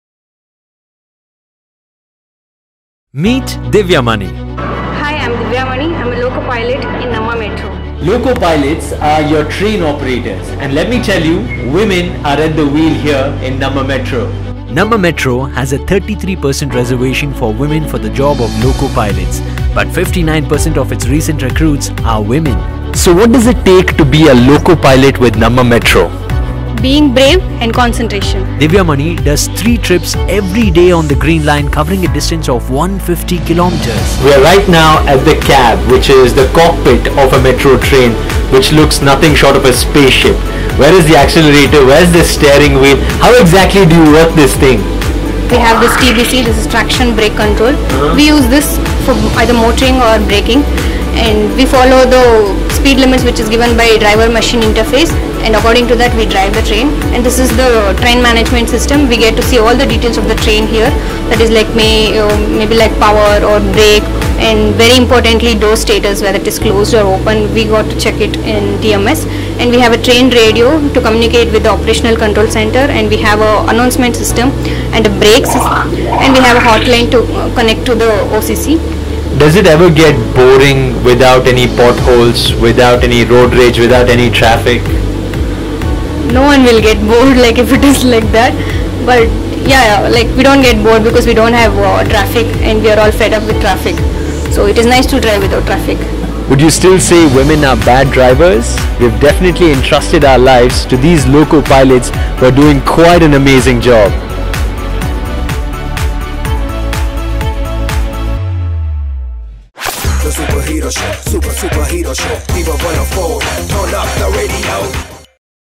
LADY DRIVERS OF NAMMA METRO
Divya Mani gave up her plush IT job so she can be a Loco-Pilot. This is just one such story behind the driving force of Namma Metro!

DO GIVE THIS A SHARE SO THE REALITY OF THE SITUATION IS CELEBRATED :)